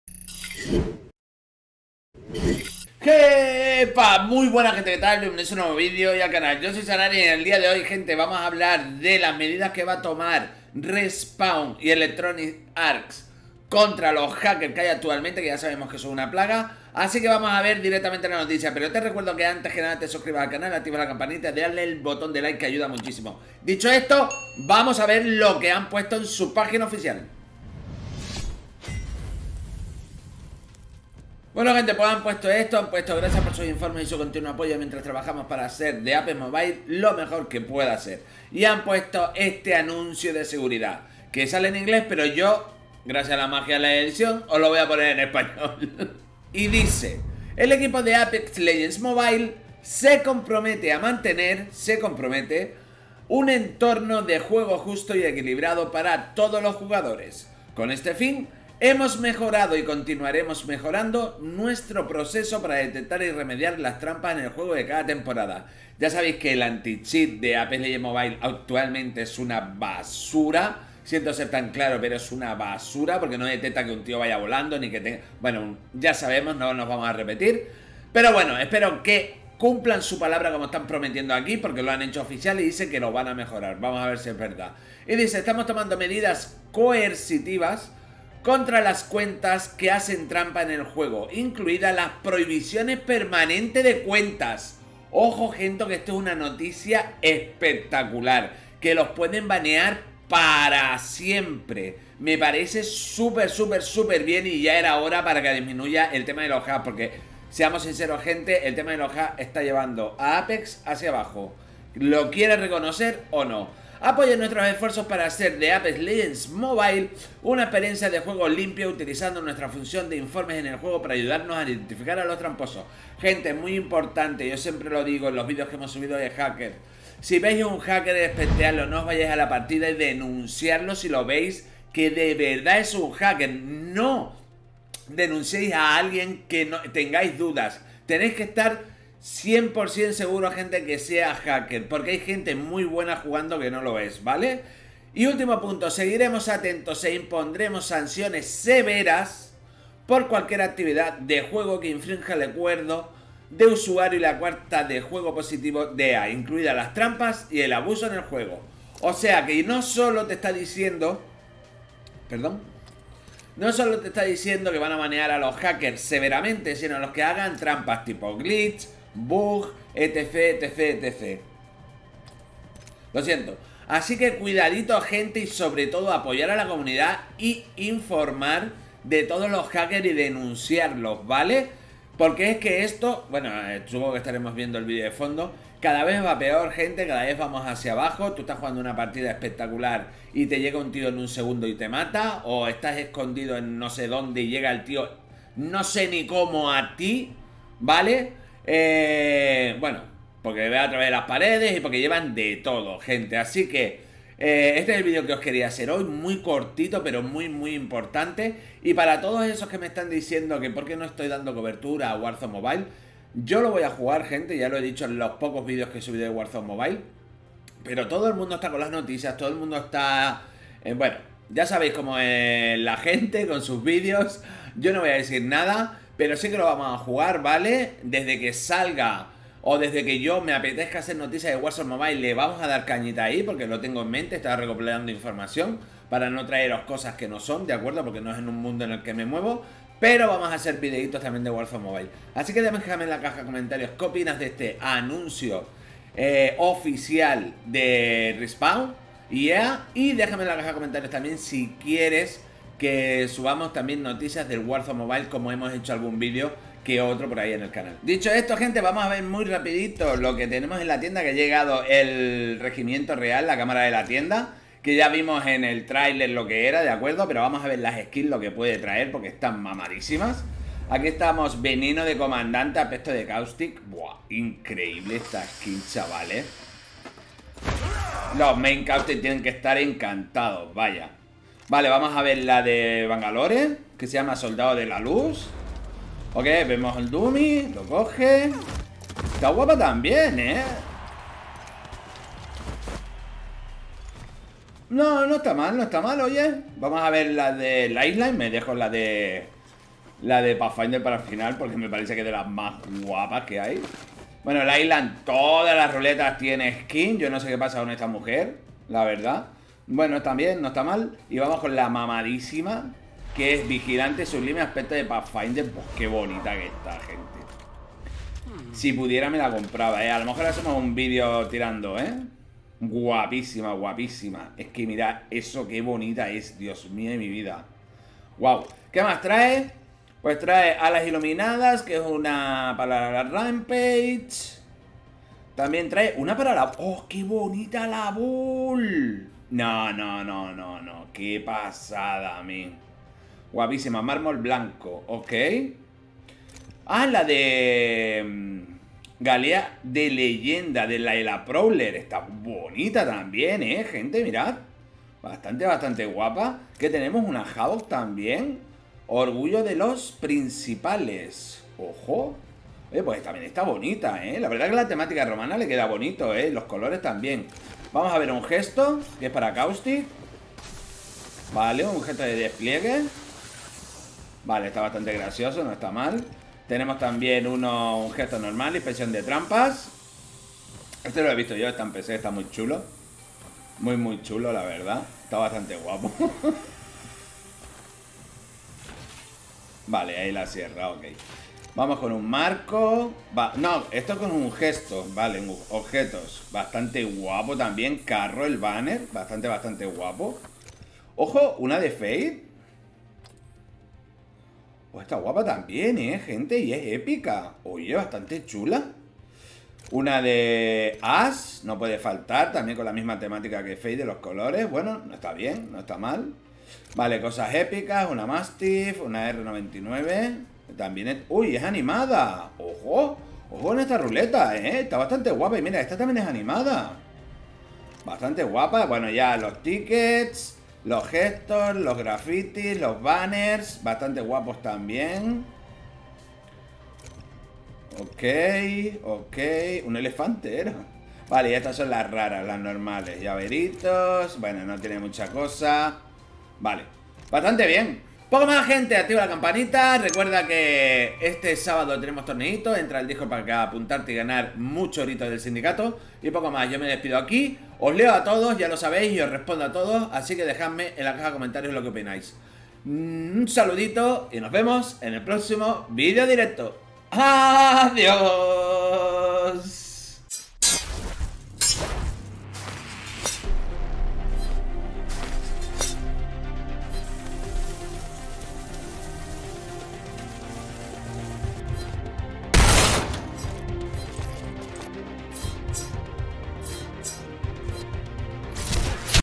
Apex Legends Mobile: RESPAWN ACTÚA CONTRA LOS HACKERS | noticia oficial ✅✅
- Comunicado oficial donde se comprometen a banear cuentas permanentemente, Respawn escucha la comunidad !! ✅ ✅

tags(ignore)
the best apex legends mobile player, the best apex legends mobile settings